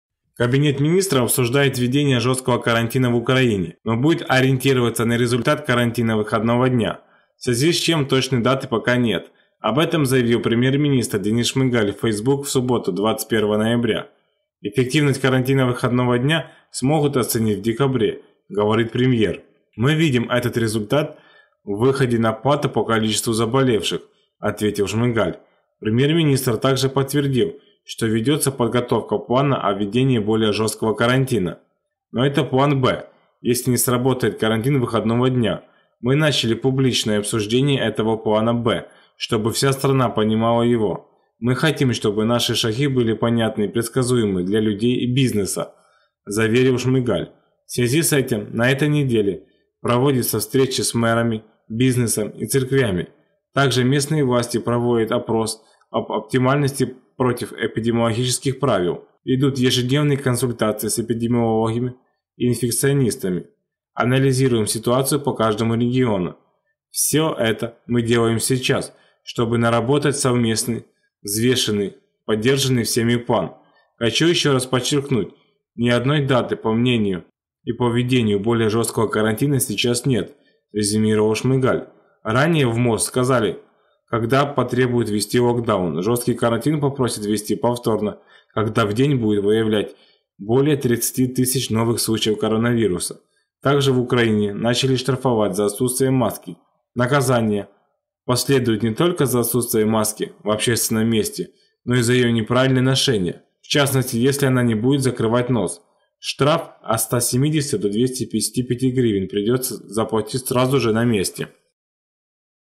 Вторая бомба для экономики Украины! Что ждет малый бизнес? Шмыгаль подтвердил подготовку к локдауну
Денис Шмыгаль подчеркнул, что жесткий карантин остается планом Б, на случай если не сработает карантин выходного дня.